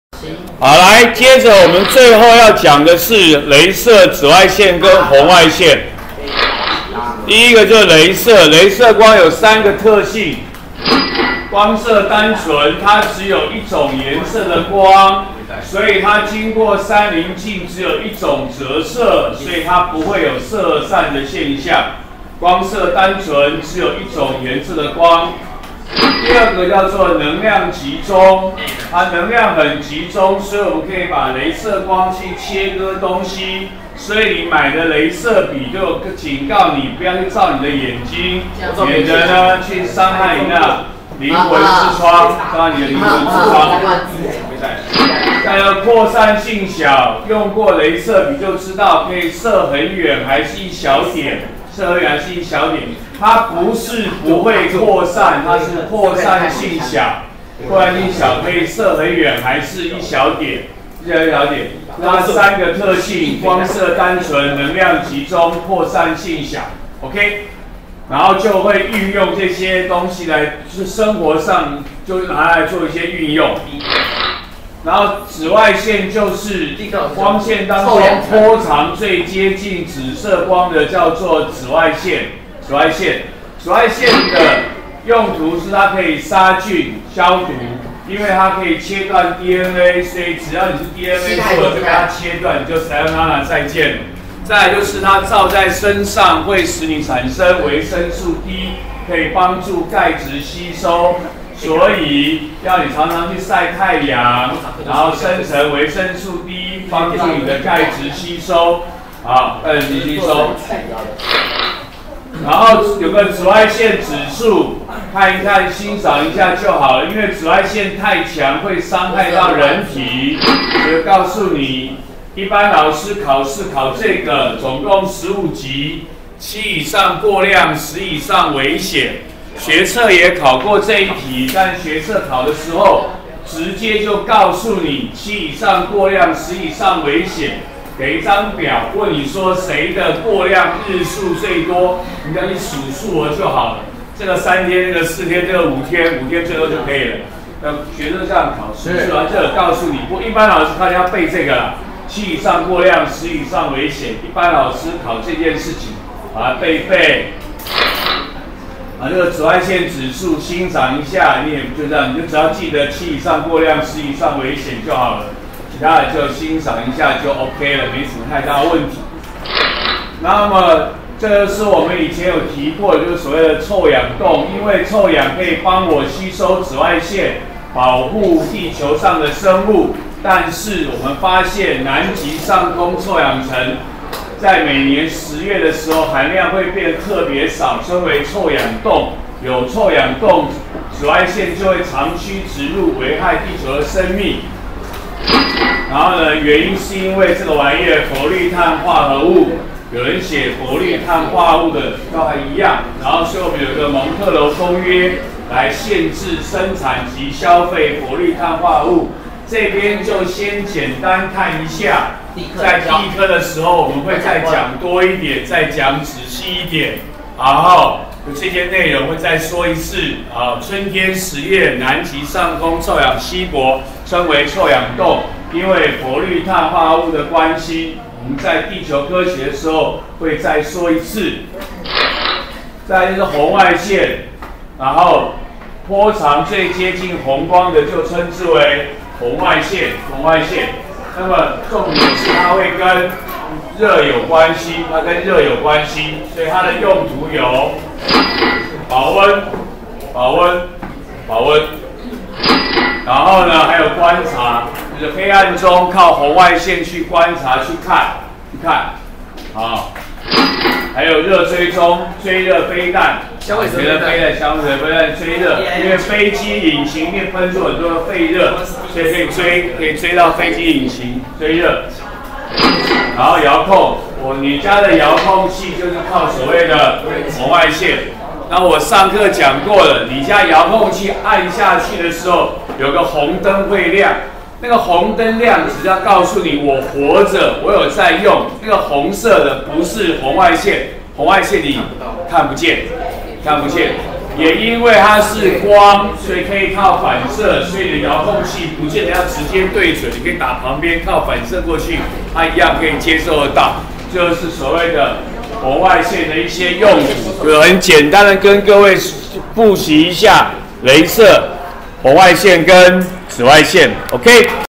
104學年複習光21：雷射；紫外線；紅外線
介紹雷射、紫外線、紅外線的性質、用途。